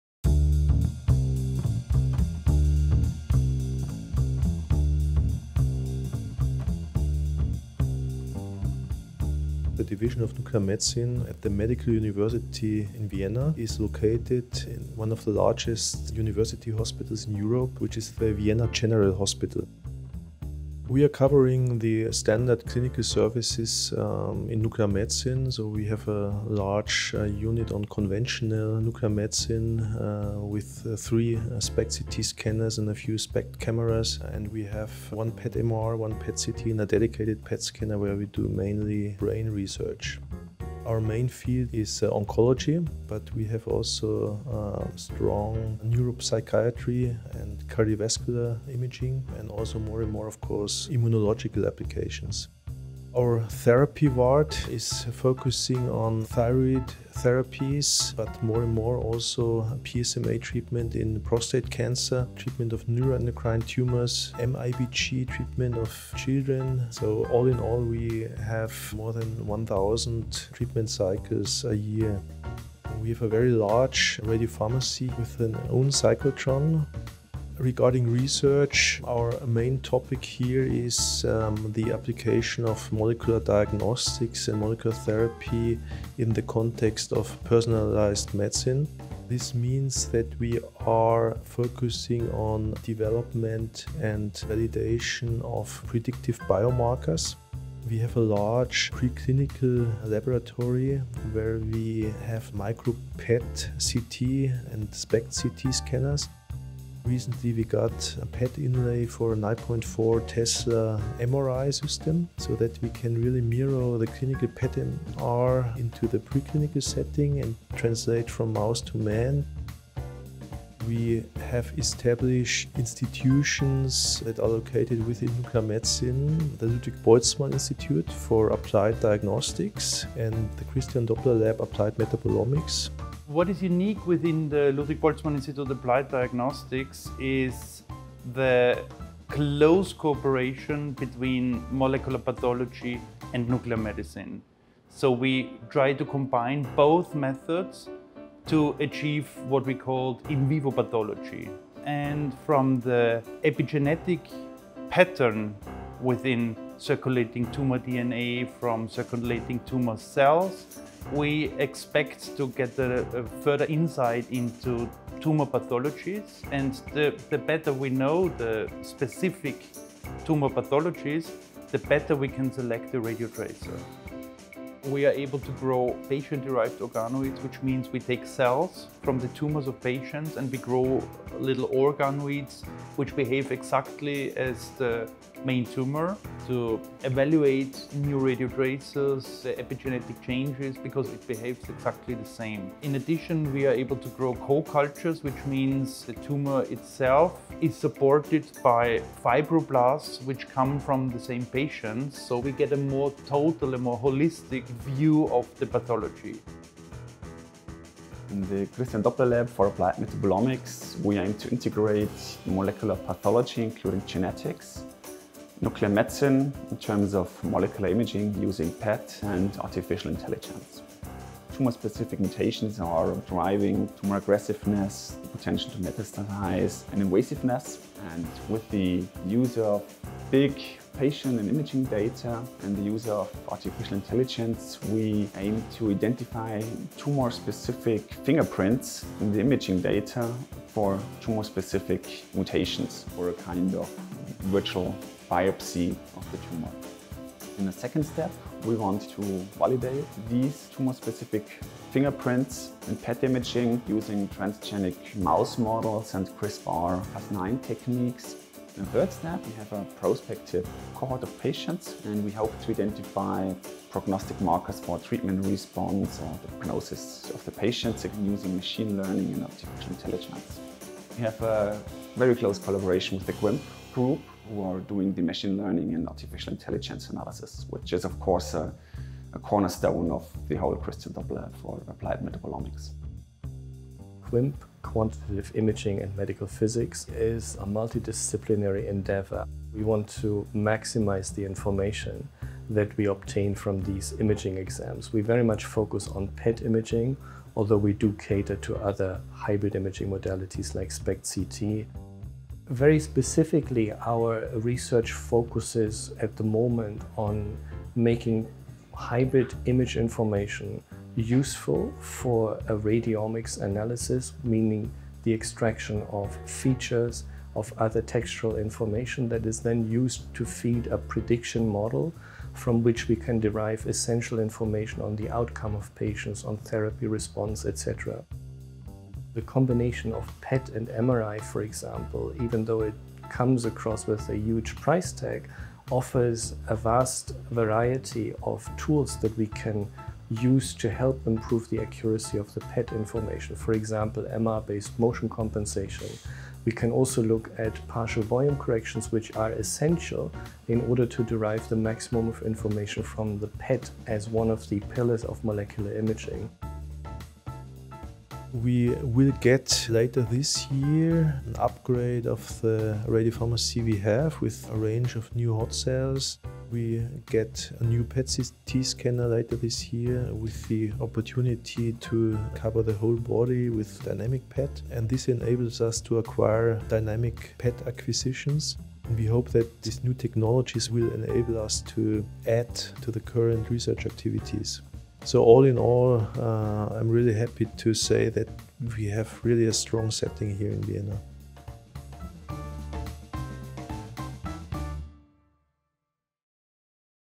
Medical University of Vienna - Division of Nuclear Medicine
The main focus of the Division of Nuclear Medicine at the Medical University Vienna is to improve the role of nuclear medicine in guiding patients towards targeted therapy as well as to predict and monitor treatment response.

The division has set up a strong biomarker development group, which in parts takes place in the context of the Ludwig Boltzmann Institute Applied Diagnostics. This institute, like many other activities, has a strong relation to molecular biology and pathology groups to enable mechanistic insights and identification of potential treatment relevant targets. Furthermore, a dual biomarker concept is developed, enabling the correlation of liquid biopsy analyses with PET and SPECT imaging for better understanding of in-vivo mechanisms and translation into the daily clinical routine.

Another nuclear Medicine Institute, the Christian Doppler Laboratory Applied Metabolism is back-translating in-vivo metabolic PET scans into underlying genetical patterns to predict actionable gene mutations of PET scans with the help of novel artificial intelligence concepts. These activities are in collaboration with the Quantitative Imaging and Medical Physics (QIMP) group, which are accompanying our activities with specific expertise in hybrid Imaging and kinetic modelling techniques.

Additional photos: (c)LBG/Schewig-Fotodesign